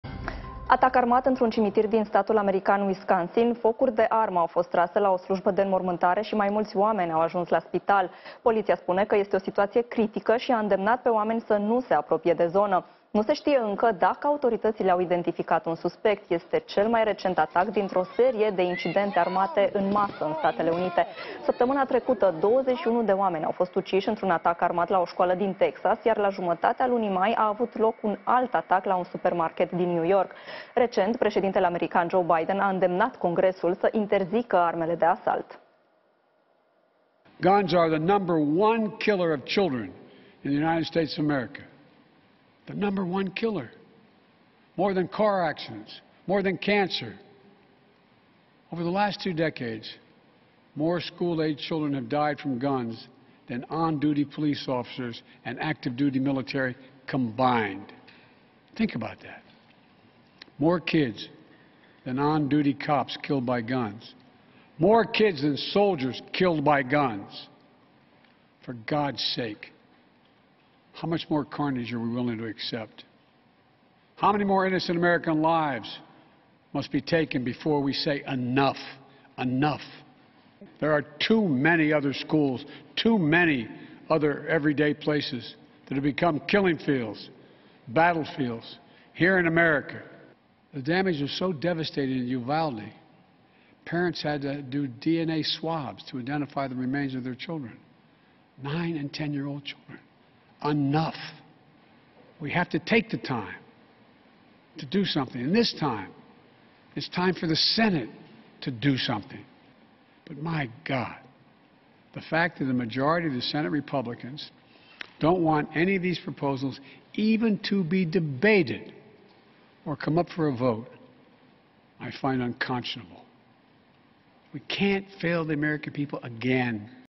Atac armat într-un cimitir din SUA, în timpul unei înmormântări. 5 oameni au fost răniți
Cinci persoane au fost rănite joi, în cursul unui atac armat într-un cimitir din Wisconsin, în nordul Statelor Unite. Incidentul s-a petrecut în timpul înmormântării unui bărbat ucis de poliţie la sfârşitul lunii mai, au potrivit polițiștilor locali.